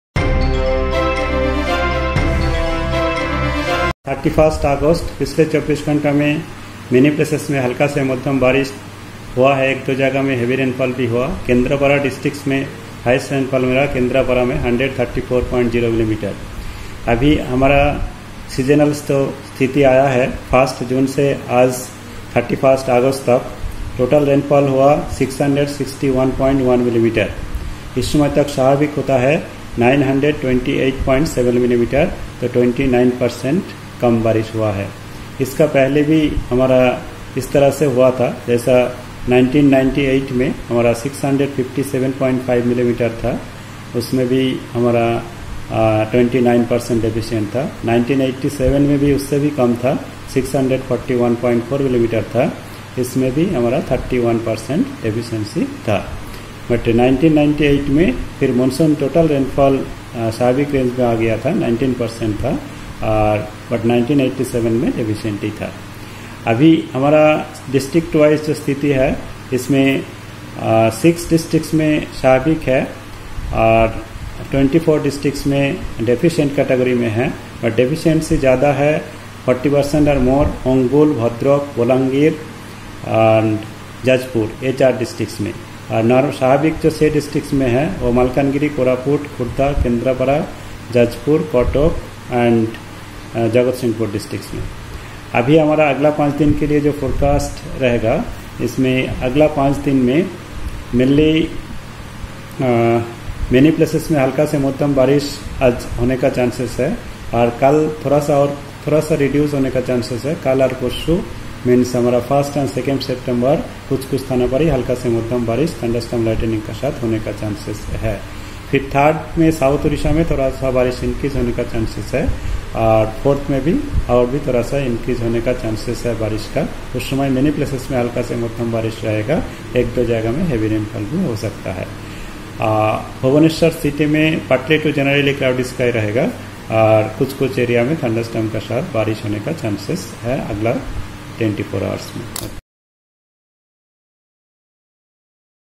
Weather briefing for next five days by Head & Scientist-E: Dr. H.R. Biswas
Weather briefing by Head & Scientist-E Dr. H.R. Biswas for next five days, based on 0830 Hrs IST observation of 31st August 2021.